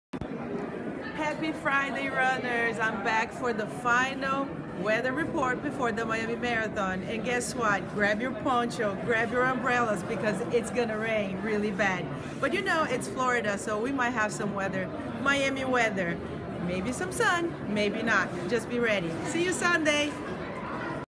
Final Weather Forecast 2 days before Fitbit Miami Marathon 2019
If you are wondering we ended up never getting any rain, it was windy, sunny hot and humid. My forecast was a big fail lol